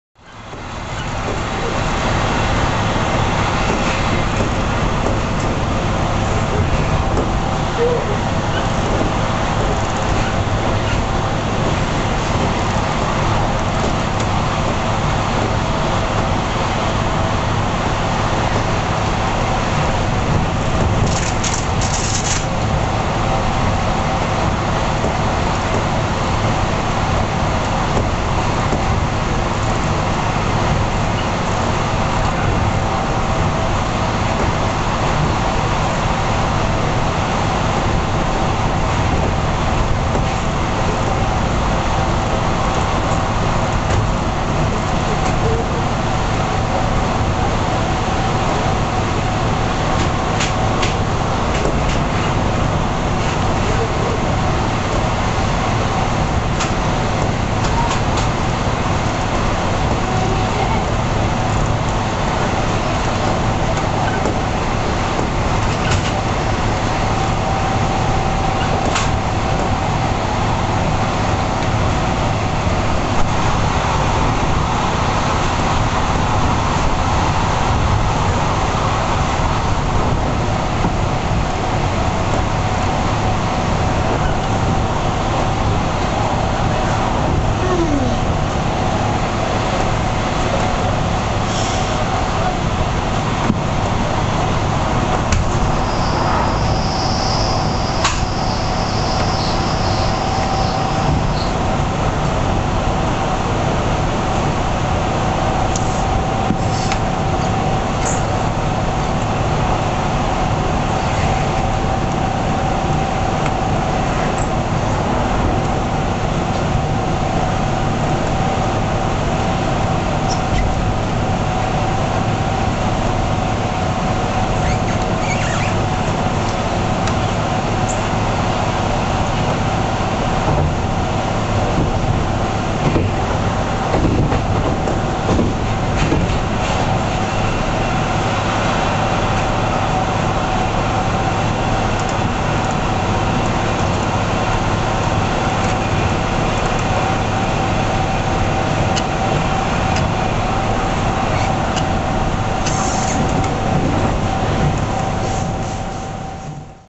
寝台特急はやぶさ 車窓 辻堂～藤沢
貫通扉にて撮影。
辻堂・藤沢間。